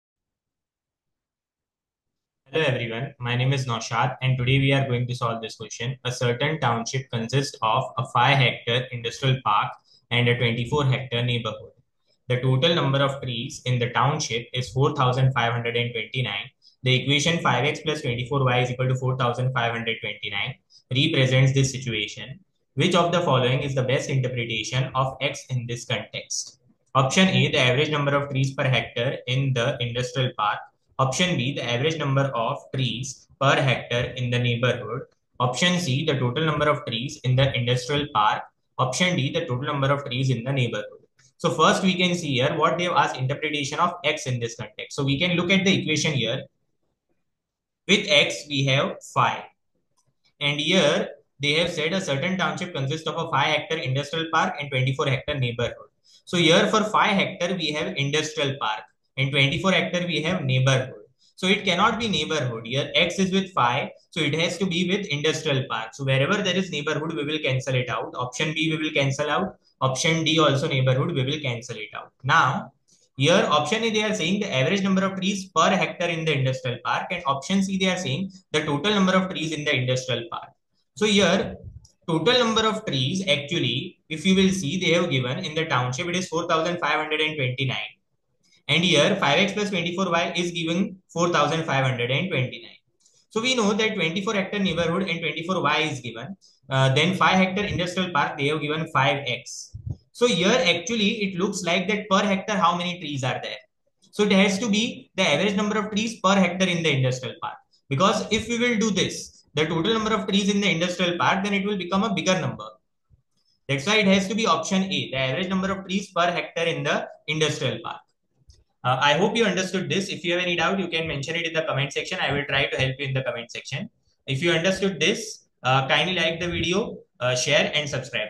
A certain township consists of a 5-hectare industrial park and a 24-hectare neighborhood. The total
A certain township consists of a 5-hectare industrial park and a 24-hectare neighborhood. The total number of trees in the township is 4,529. The equation 5x + 24y = 4,529 represents this situation. Which of the following is the best interpretation of x in this context?
(A) The average number of trees per hectare in the industrial park
(B) The average number of trees per hectare in the neighborhood
(D) The total number of trees in the neighborhood

Bluebook Digital SAT Practice Test 9 Math Module 1 Question 14